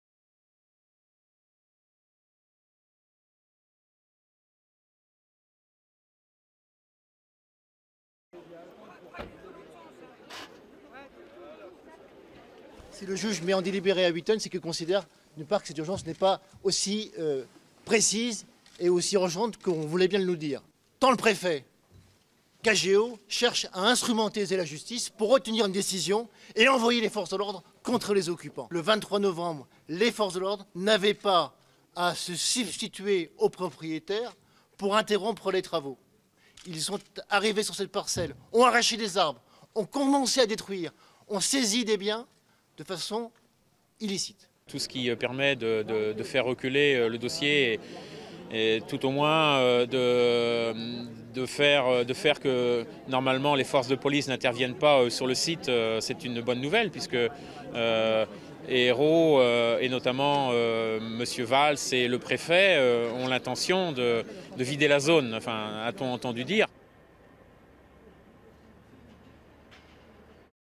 Notre-Dame-des-Landes: la destruction des cabanes en délibéré
Le juge des référés de Saint-Nazaire a indiqué mardi qu'il ne rendrait que le 11 décembre sa décision sur la demande de démolition des nouvelles cabanes des opposants à l'aéroport de Notre-Dame-des-Landes. Durée: 01:10